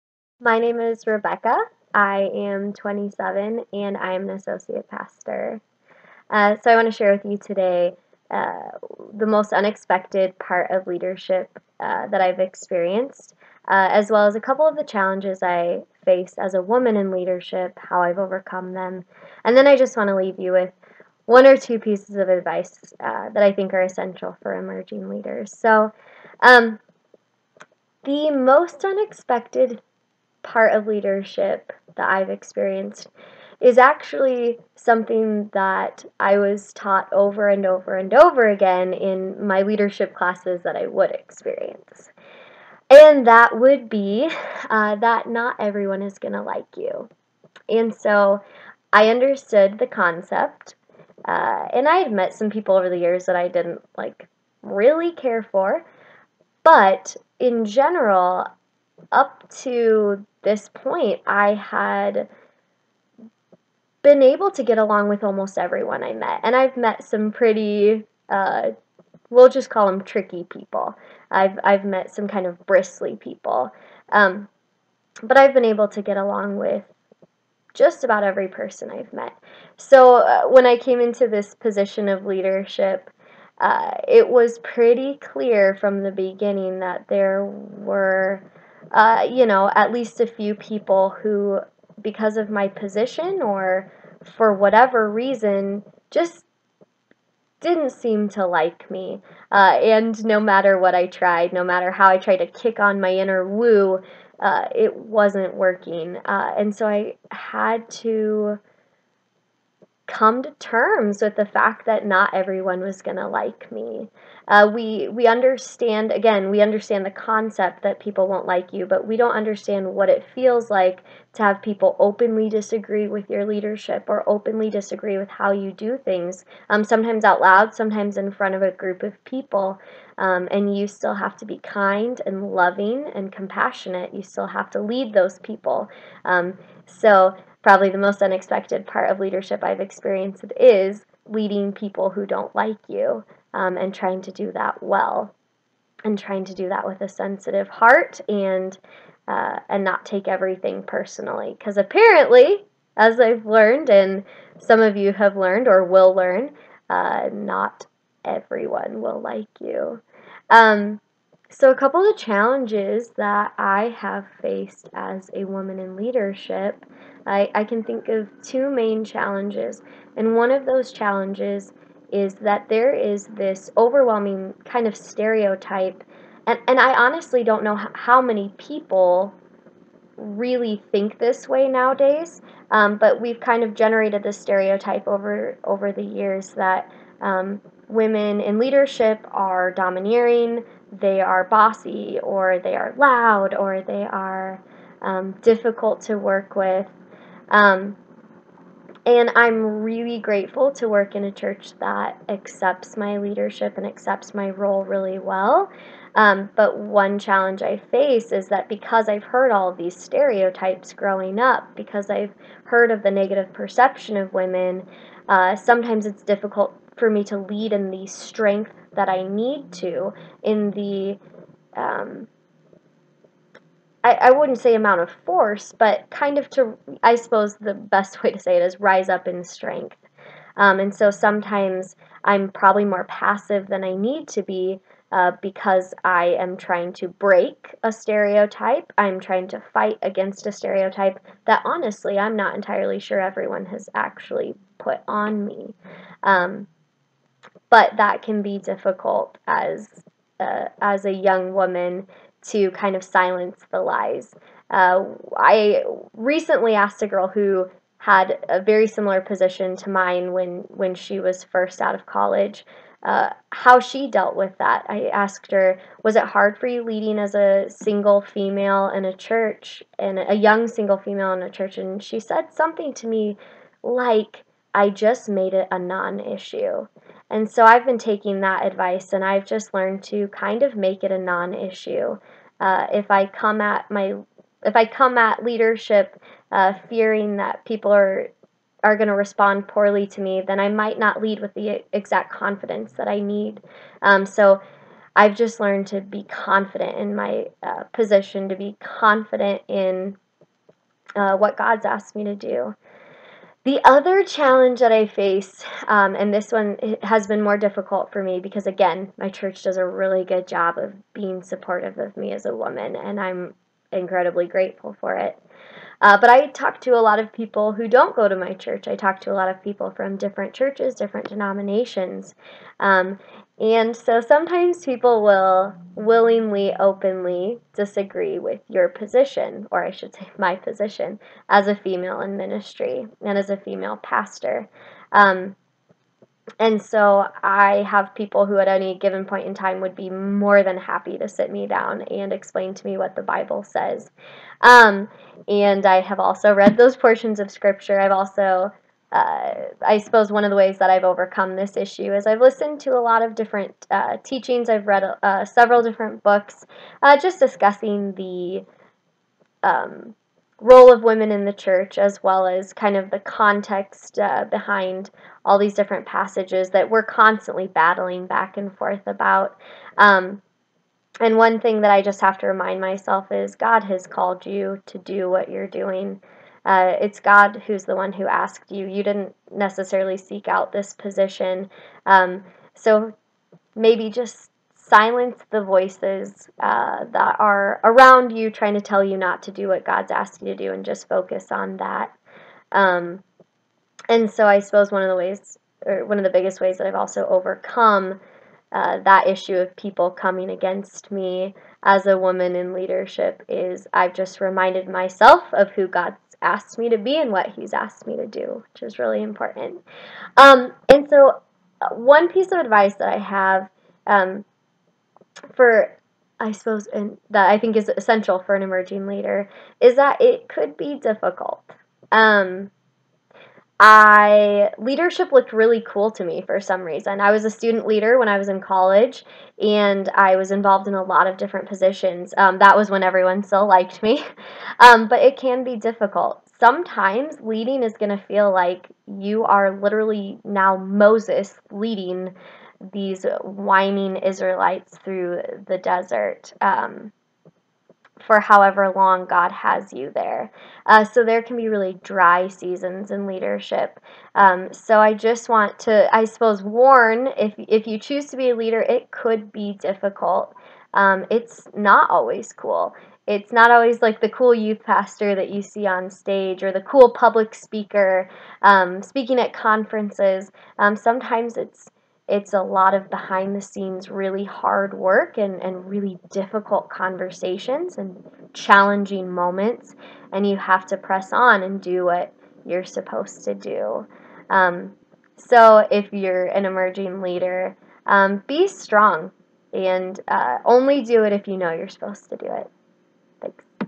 Rebecca Pleuler - 25-40 Interview
Rebecca Pleuler is the associate pastor at Omaha Christian Center in Omaha, NE.